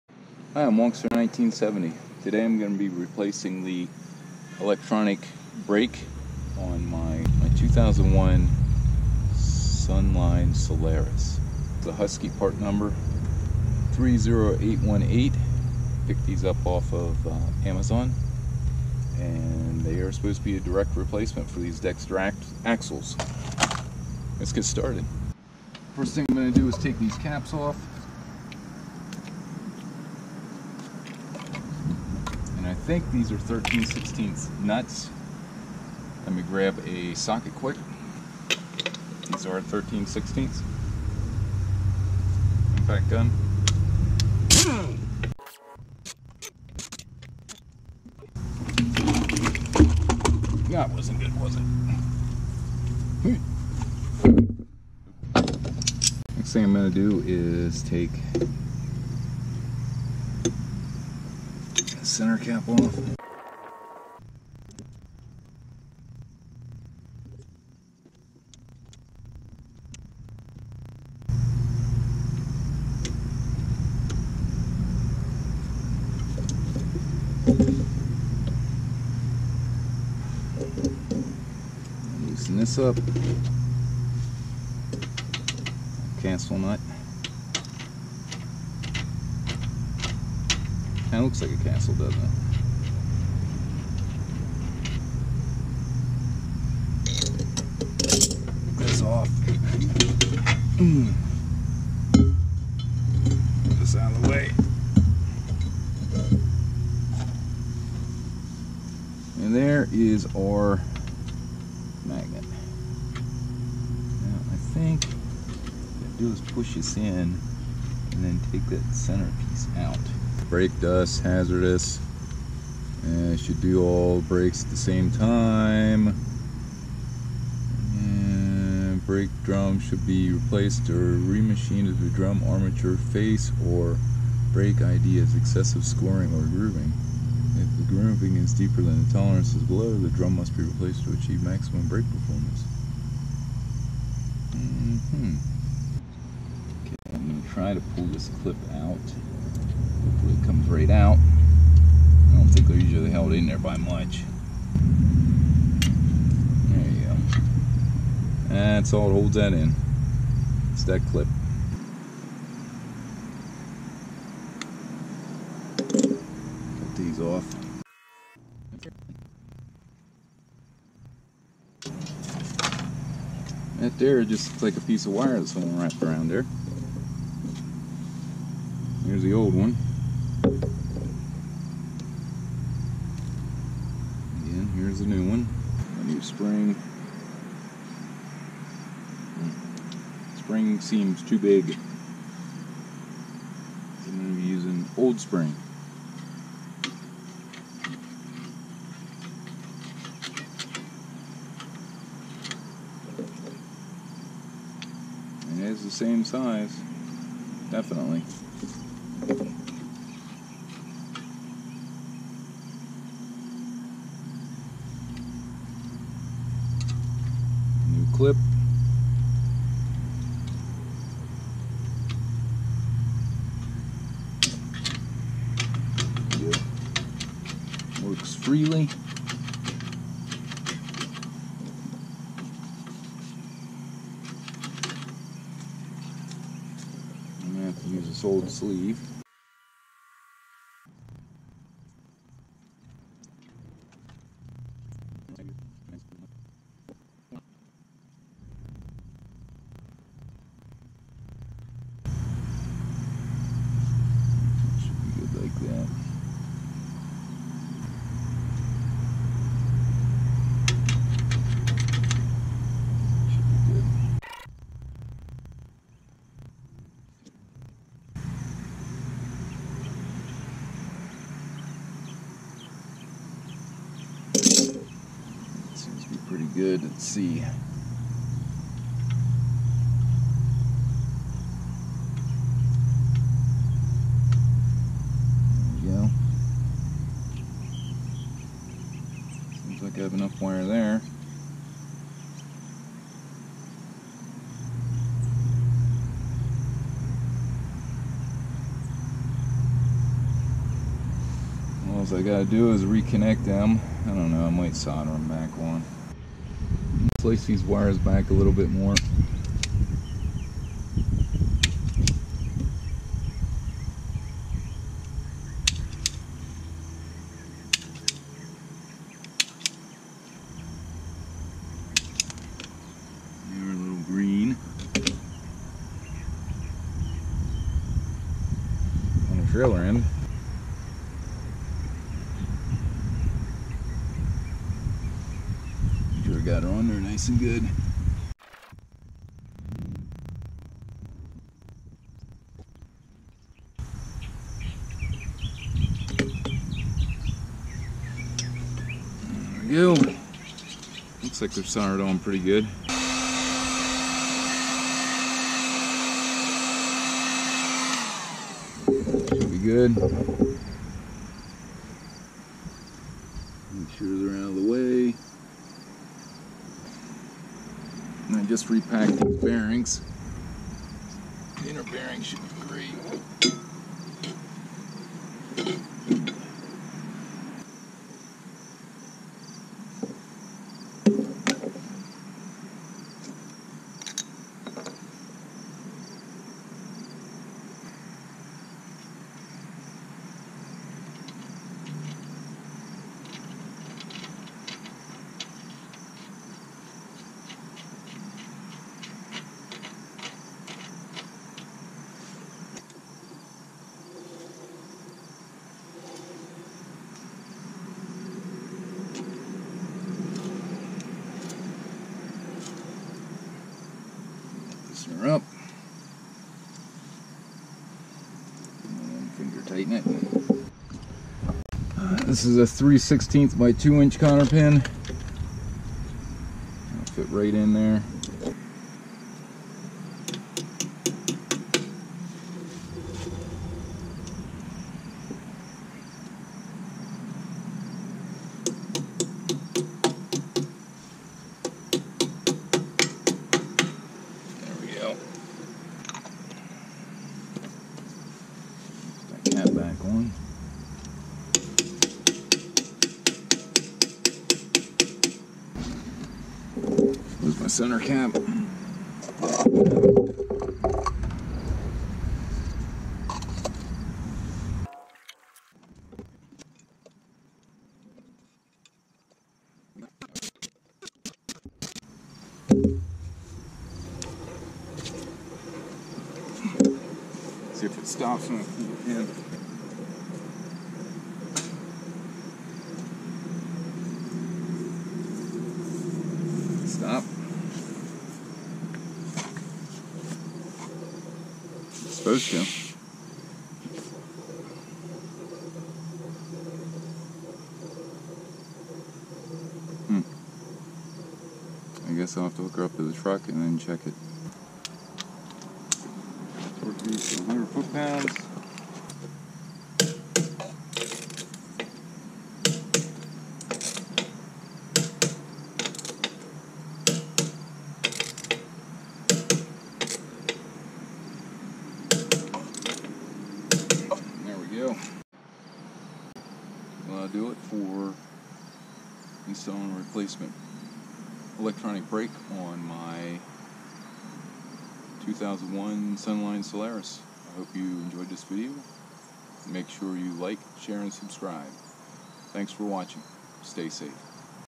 2001 Sunline Solaris Magnetic Brake Replacement
In this video, I replace the right rear brake magnet on my 2001 Sunline Solaris trailer. On the way back from our last camping excursion, I noticed that the brake drum was hot. I had previously repacked all of the bearings and replaced the seals. Upon further investigation, I discovered that the brake was dragging. The magnet was in constant contact with the drum. The brake drum has been operating much cooler after installing the new magnet. Make sure that you check your brakes after repair by pulling the brake-away while rotating the wheel. The brakes should engage immediately. Stay safe!